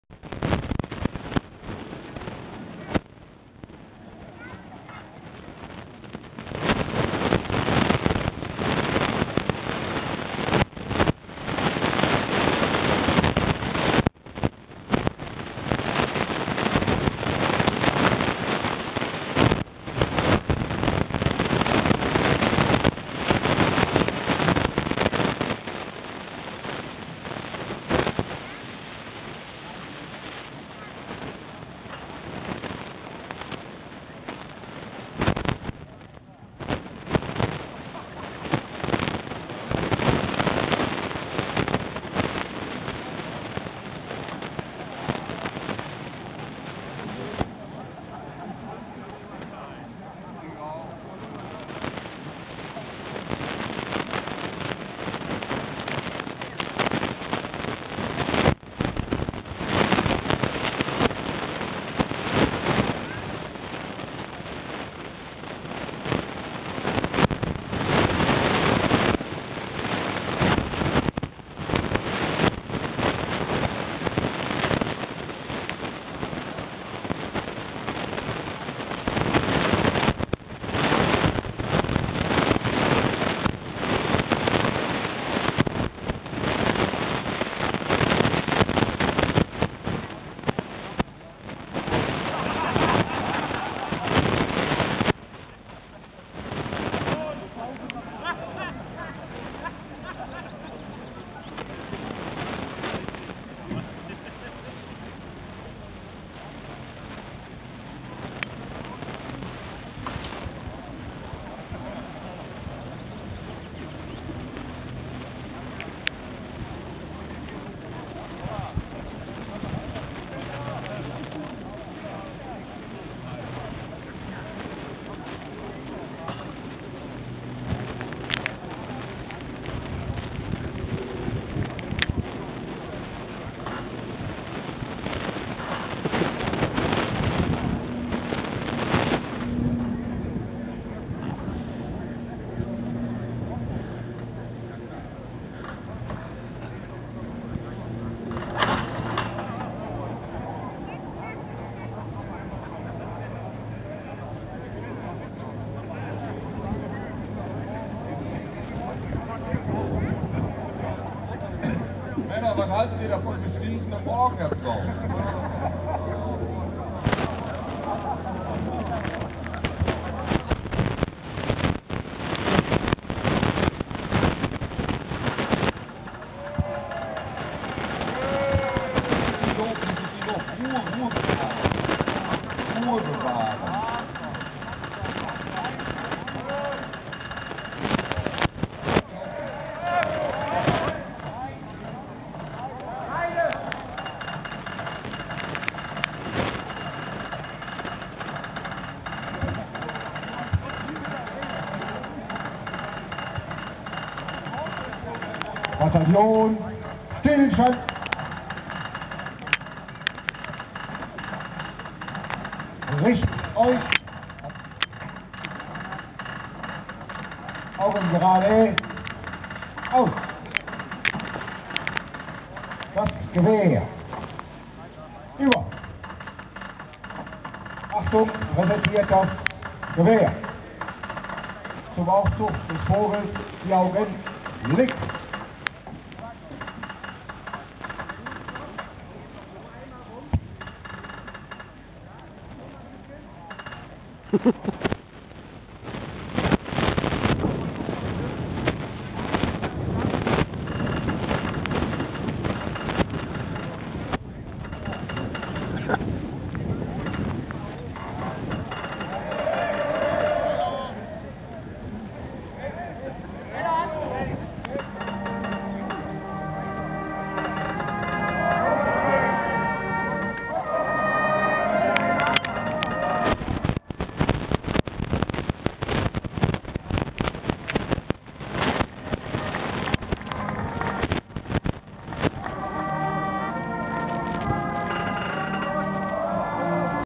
Technische Panne Schützenfest Bottrop
War ein Vogel aus Holz,
gab sich gar zu Stolls.
und wollte der Flinte Lauf nicht gefallen,
drum neigte er zum fallen.

Doch mit der Fröhlicheit der Mannen,
ließ sich der Piepmatz bannen,
und wurde zu guter letzt,
vom dem Kaliber zerfest.